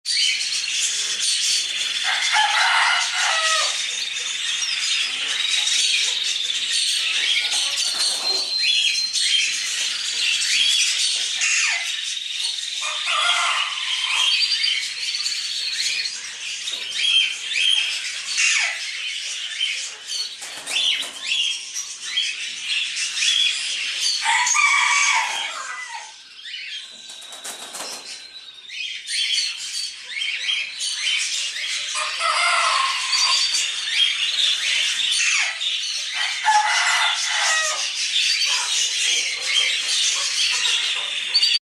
オニオオハシ入荷しました(PETSUN CO.,LTD)
南米産のオニオオハシです。